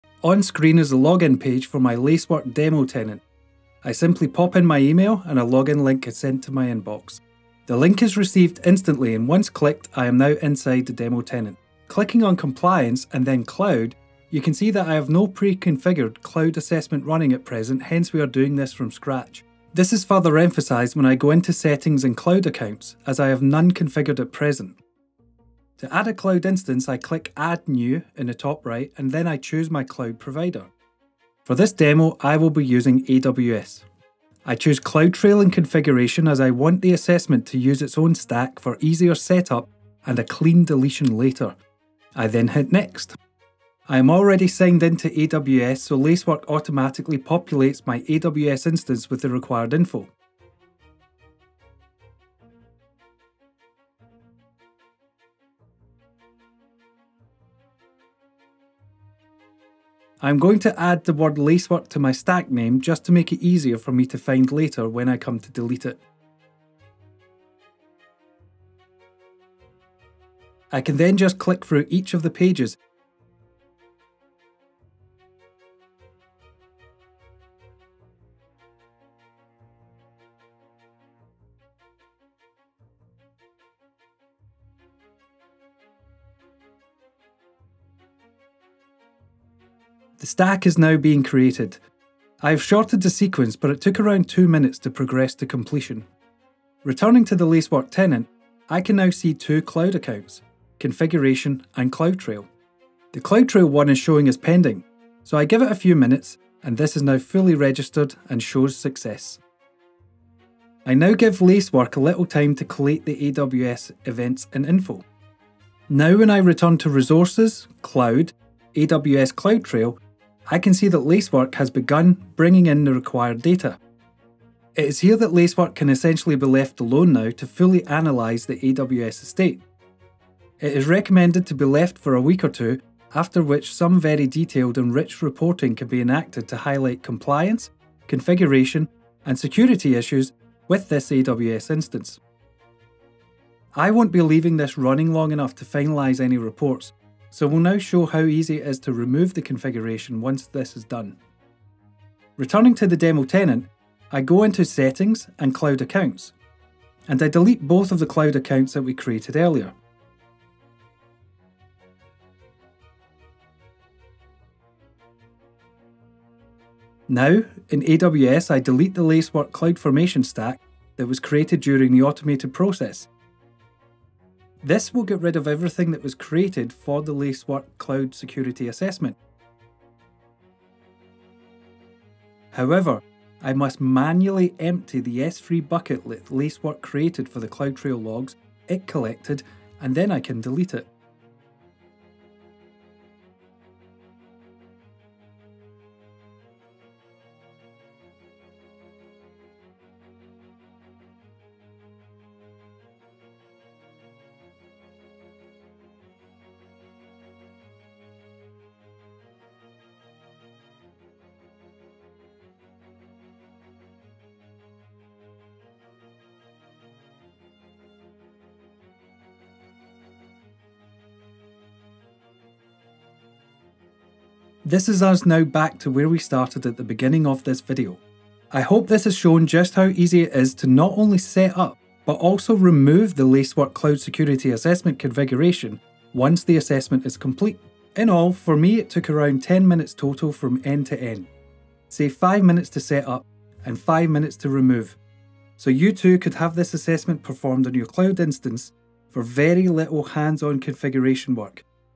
Lacework Cloud Security Assessment (CSA) Demo | Automating Security Assessments with Lacework
Join Somerford for a short video introduction and demonstration of the Lacework Cloud Security Assessment (CSA), and how it enables organisations to automate security assessments with the power of the Lacework platform.